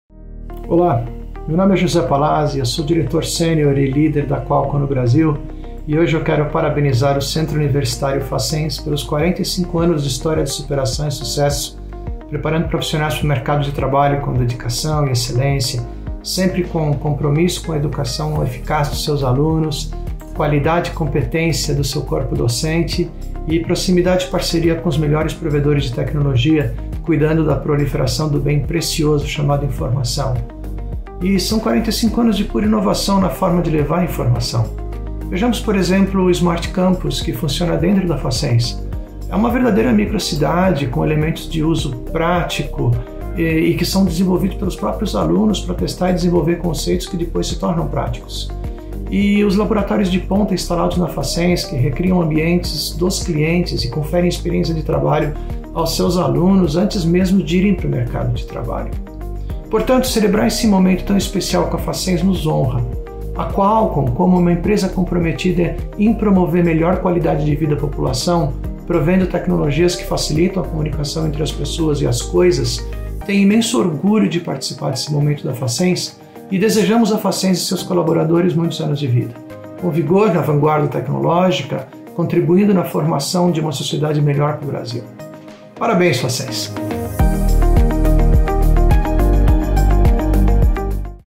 45 anos Facens - José Palazzi - Diretor Sênior Qualcomm no Brasil
José Palazzi, Diretor Sênior Qualcomm no Brasil, também está comemorando os 45 anos de Facens e mandou um recado especial para nós!

Obrigada pela parceria, Palazzi!